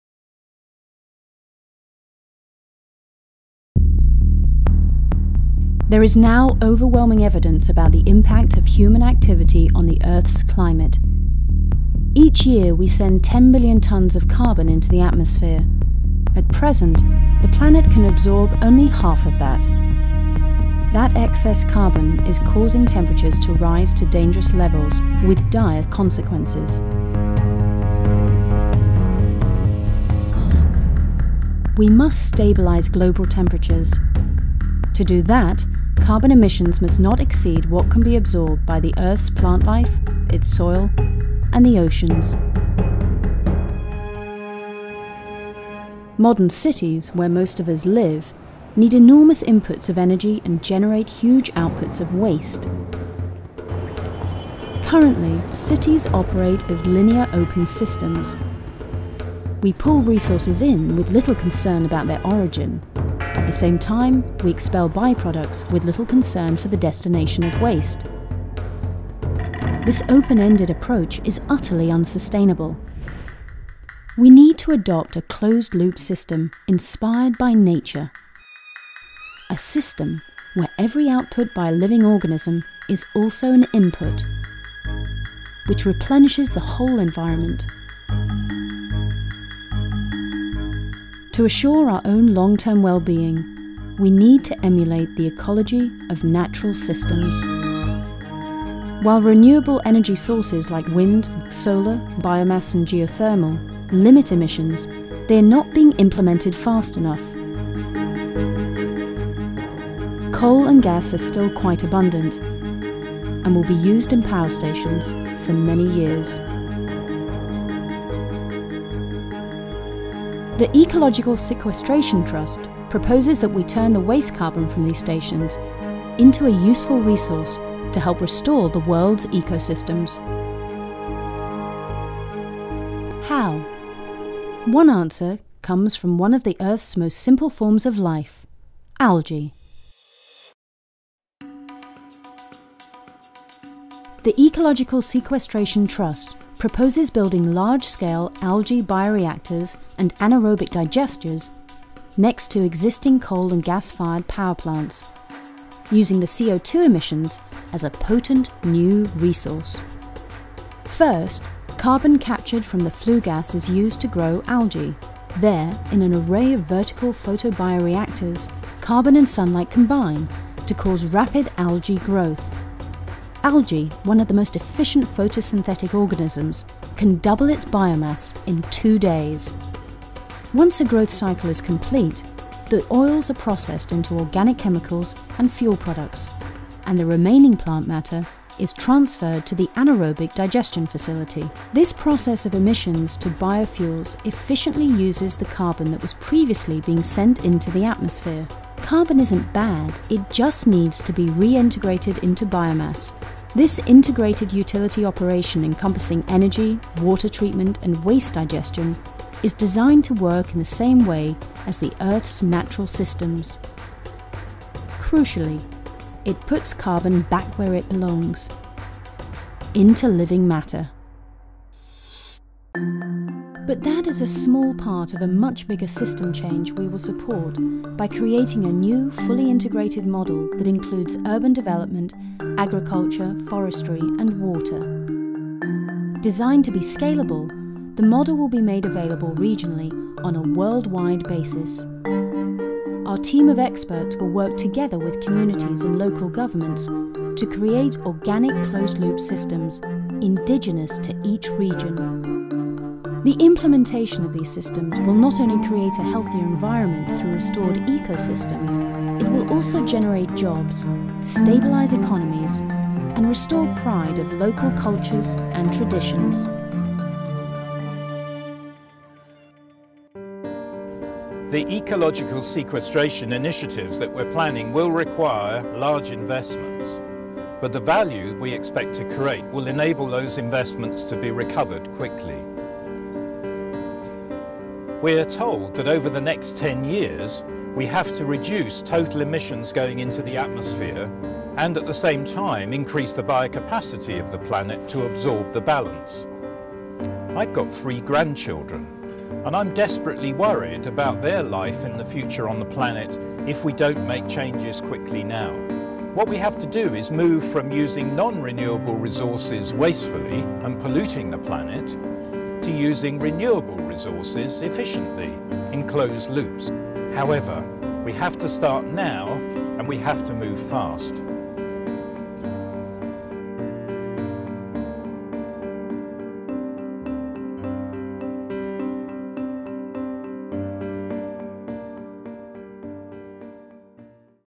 Introduction - The Ecological Sequestration Trust
There is overwhelming evidence that human activity is affecting the Earth's climate.

Each year we send 10 billion tons of carbon into the atmosphere.  At present the planet can absorb only half of that. That excess carbon is causing temperatures to rise to dangerous levels -- with the potentially catastrophic effects.

We must stabilize global temperatures. To do that, carbon emissions must not exceed what can be absorbed by the Earth's plant life, its soil and the oceans.

Modern cities, where most of us live, need enormous inputs of energy and generate huge outputs of waste. Currently cities operate as linear, open systems. We pull resources in with little concern about their origin. At the same time, we expel by-products with little concern for the destination of waste. This open-ended approach is utterly unsustainable.

We need to adopt a closed loop system inspired by nature. A system where every output by a living organism is also an input  which replenishes the whole environment. To assure our own long-term well-being, we need to emulate the ecology of natural systems.

While renewable energy sources like solar, wind, biomass and geothermal limit emissions, they are not being implemented fast enough. Coal and gas are still quite abundant, and will be used in power stations for many years.

The Ecological Sequestration Trust proposes that we turn the waste carbon from these stations into a useful resource to help restore the world's ecosystems.

Responding to this climate and resource challenge will require radical transformation in how we run our economies, societies and lives. Without this, we will not be able to secure energy, food, and water security for future generations.